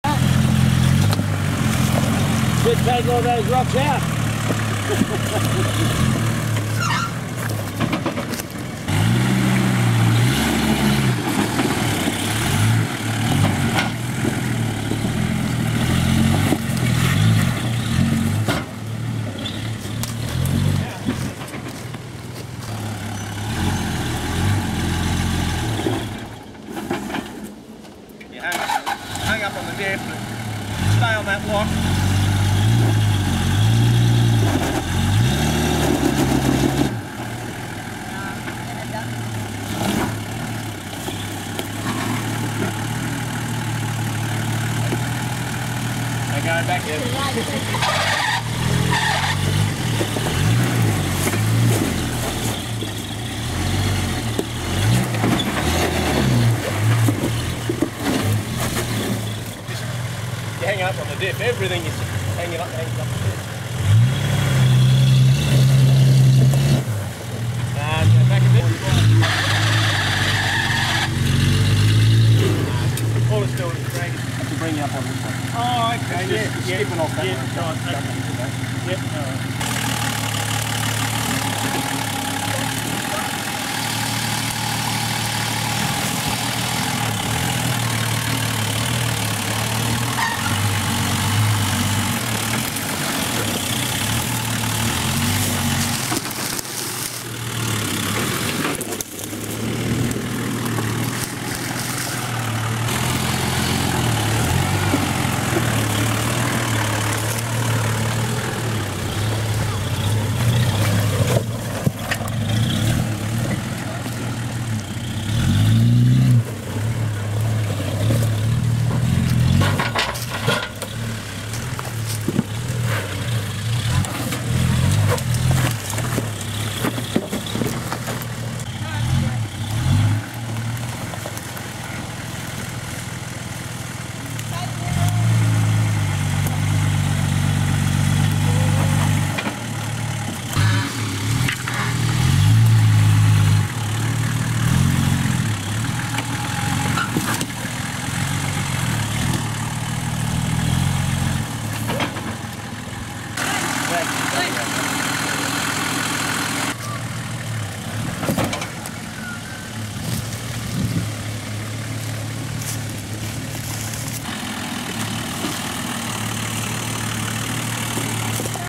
60 series attempts Ourimbah rock garden
60 series Toyota Landcruiser attempts the rock garden in Ourimbah State Forest.
Part of a Dust 'N Dirt club drive.